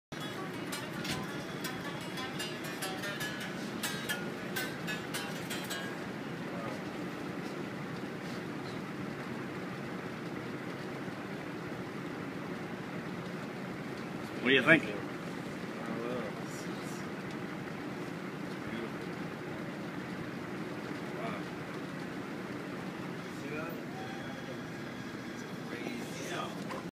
Gary Clark Jr
Gary receives his one of a kind Crash Kantor Guitar!